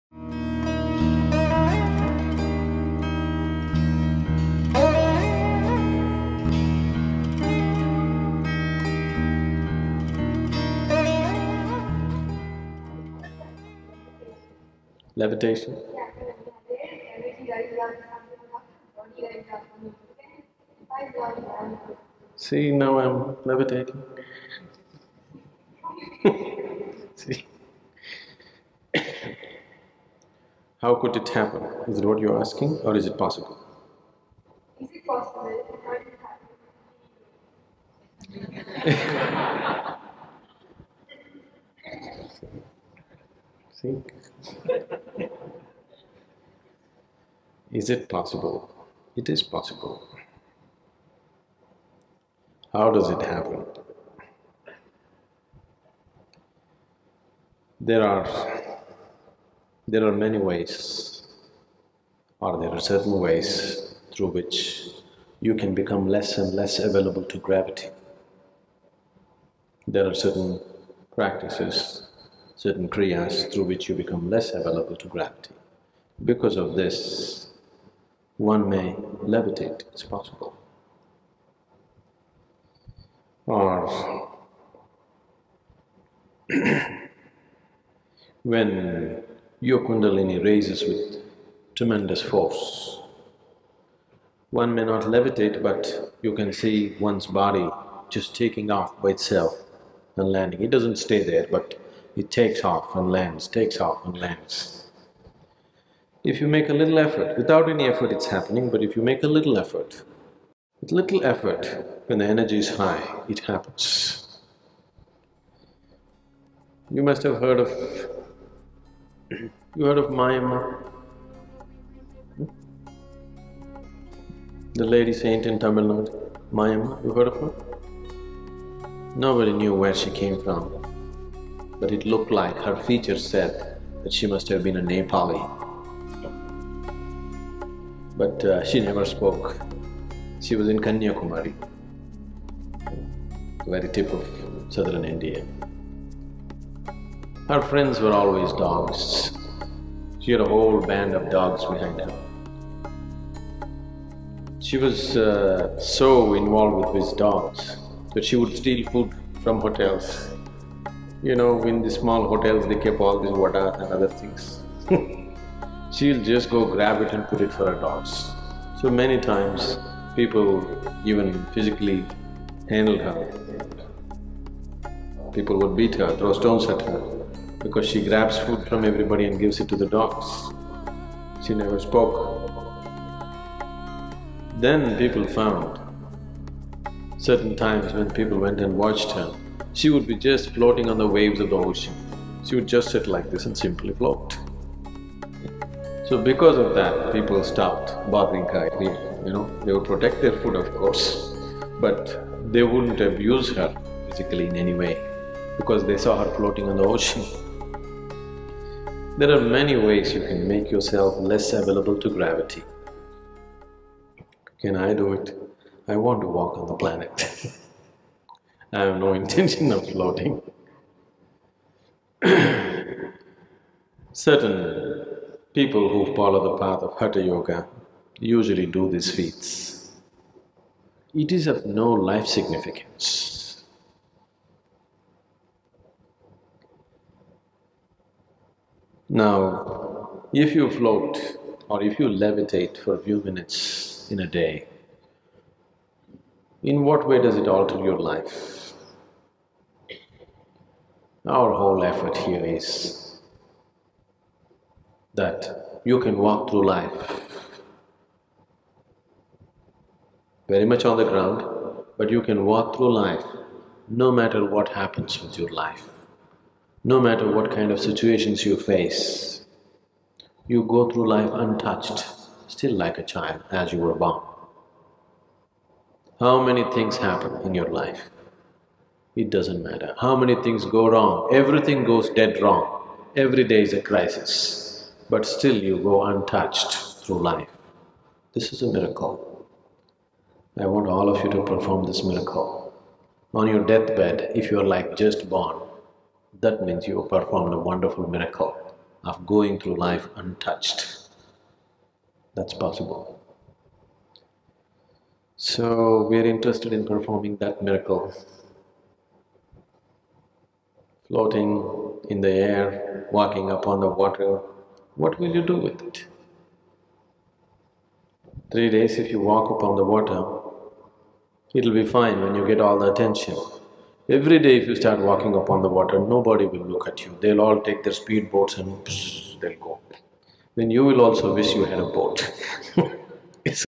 Can Humans Really Levitate?
Is it possible to become less available to gravity and levitate? Sadhguru gives us his perspective.